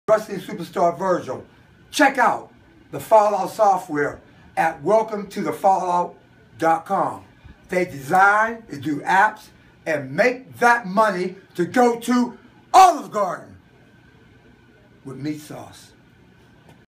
Virgil making that money
virgil taking about the fallout software and making that money